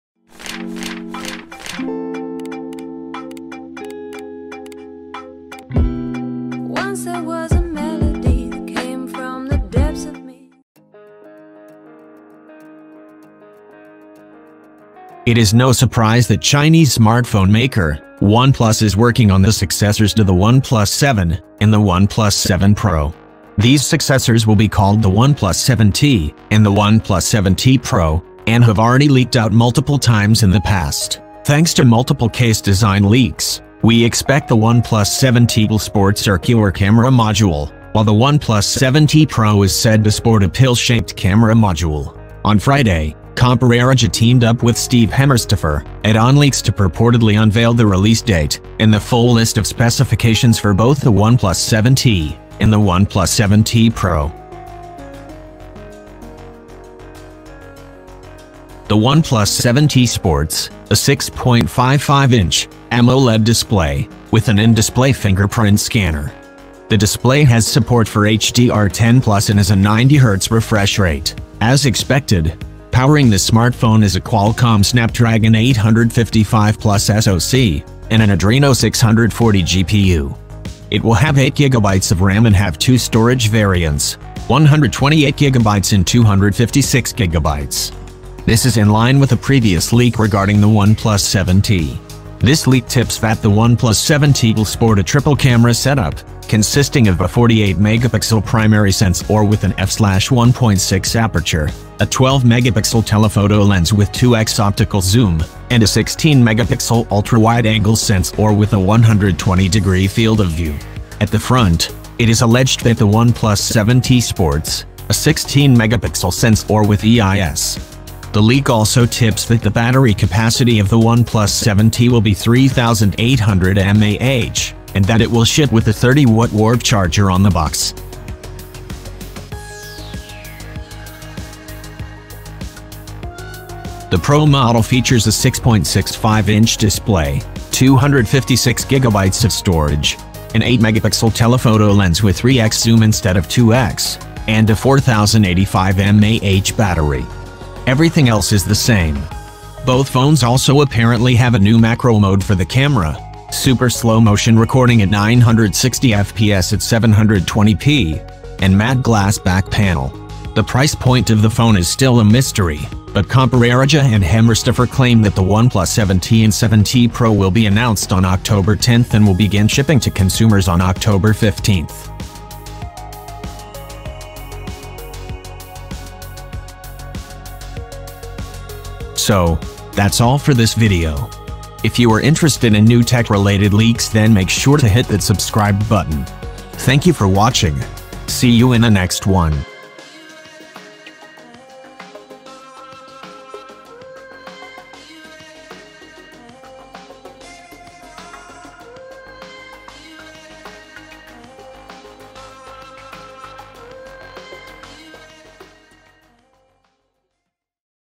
OnePlus 7T & 7T Pro: Everything You Wish to Know | Specs, Release Date | LEAK!!!
In this video you will be getting all details about OnePlus 7T & 7T Pro: Everything You Wish to Know | Specs, Release Date | LEAK!!!
As OnePlus 7T & 7T Pro: Everything You Wish to Know | Specs, Release Date | LEAK, It will also apparently have an in-screen fingerprint scanner, a Snapdragon 855 Plus chipset, 8GB of RAM, 256GB of storage, and a triple-lens rear camera, with a 48MP f/1.6 main lens, an 8MP f/2.4 telephoto lens (with 3x optical zoom), and a 16MP f/2.2 ultra-wide lens (with a 120-degree field of view).

Comments will be read and please post feedback on videos.